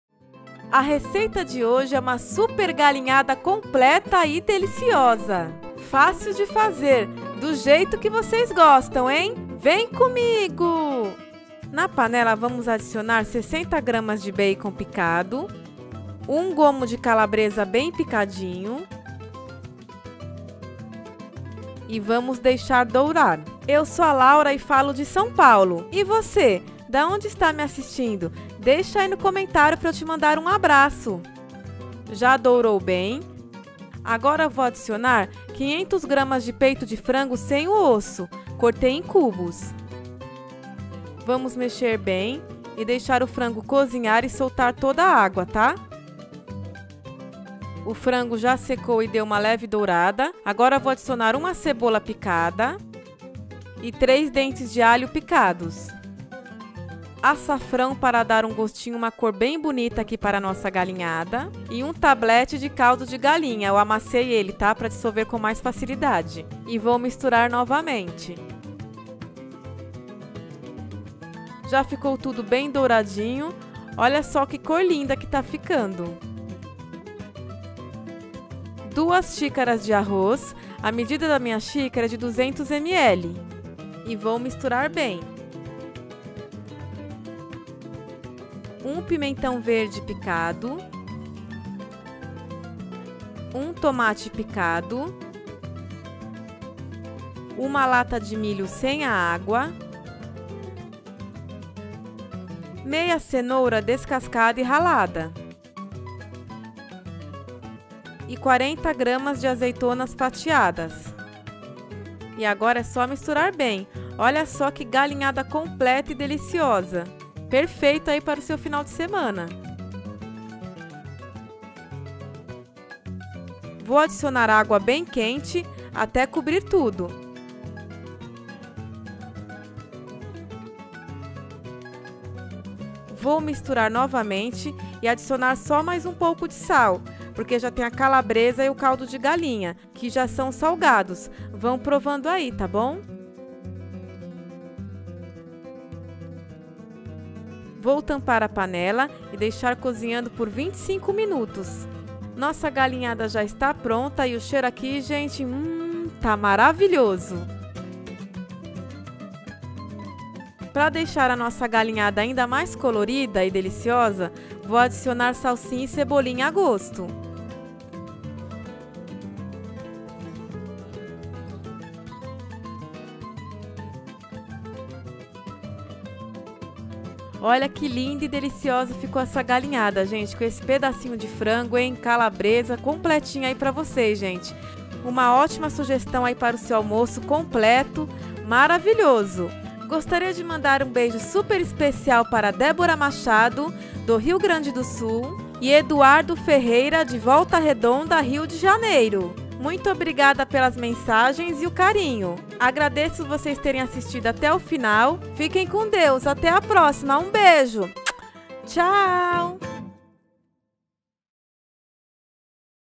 GALINHADA COM ARROZ - REFEIÇÃO COMPLETA E FÁCIL DE FAZER
Galinha com arroz - Refeição completa e fácil de fazer

✅INGREDIENTES:

- 60g de bacon
- 1 gomo de calabresa
- 500g de peito de frango sem o osso
- 1 cebola picada
- 3 dentes de alho picados
- açafrão
- 1 tablete de caldo de galinha
- 2 xícaras de arroz
- 1 pimentão verde picado
- 1 tomate picado
- 1 lata de milho sem a água
- 1/2 cenoura descascada e ralada
- 50g de azeitonas fatiadas